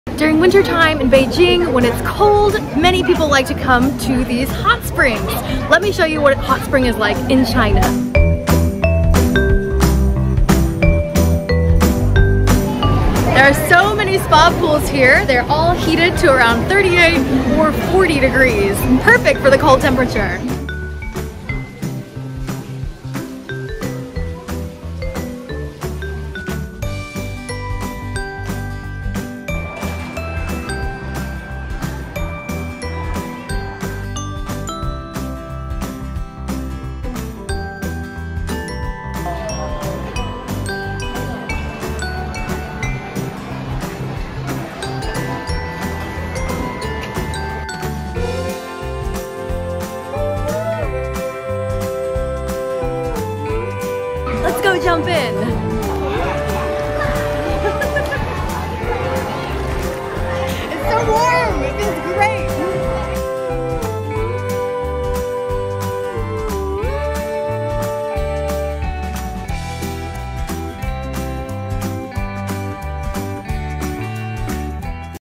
Hot springs a popular winter activity in Beijing
During the wintertime in Beijing, many people find fun and relaxing ways to stay warm such as visiting a hot spring! Hot springs usually have outdoor heated pools, hot stones, buffets, massages, and many other elements to relax and pamper visitors. Follow Rachel as she checks out a local Beijing hot spring.